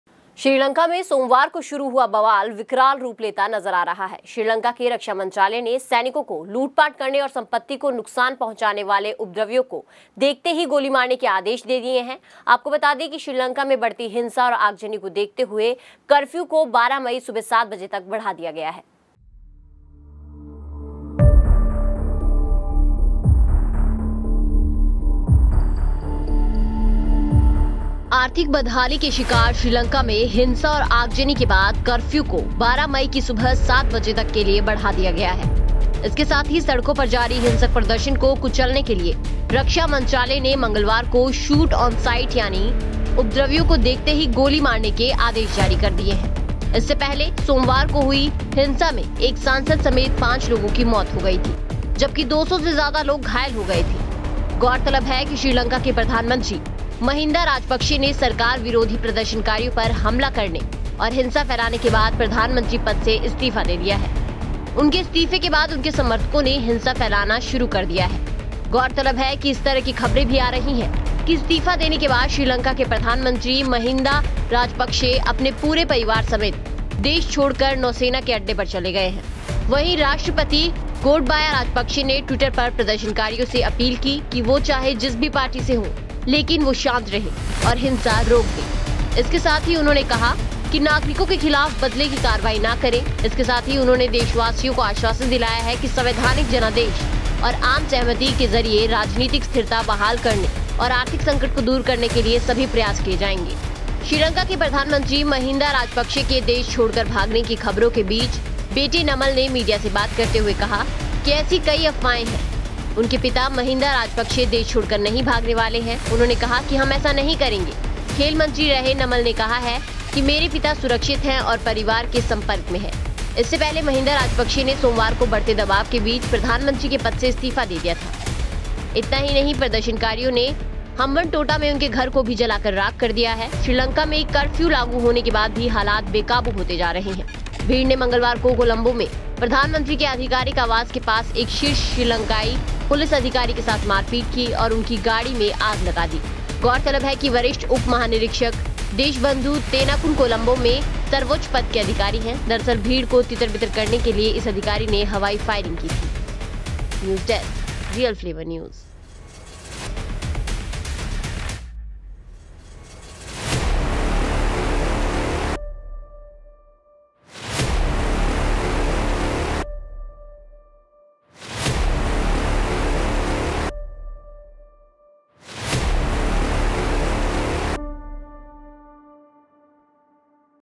Sri Lanka crisis: बद से बदतर हुए हालात, दंगाइयों को देखते ही गोली मारने के आदेश
श्रीलंका में सोमवार को शुरू हुआ बवाल अब विकराल रूप लेता जा रहा है। खबरों के मुताबिक श्रीलंका के रक्षा मंत्रालय ने मंगलवार को सैनिकों को लूटपाट करने वालों और संपत्ति को नुकसान पहुंचाने वाले उपद्रवियों पर गोली चलाने के आदेश दे दिए हैं।श्रीलंका में हिंसा और आगजनी के बाद कर्फ्यू को 12 मई की सुबह सात बजे तक के लिए बढ़ा दिया गया है.

The ruckus that started in Sri Lanka on Monday is now taking a formidable form. According to reports, the Defense Ministry of Sri Lanka has ordered soldiers to open fire on miscreants who looted and damaged property. After violence and arson in Sri Lanka, the curfew was extended till 7 am on May 12. Has been given.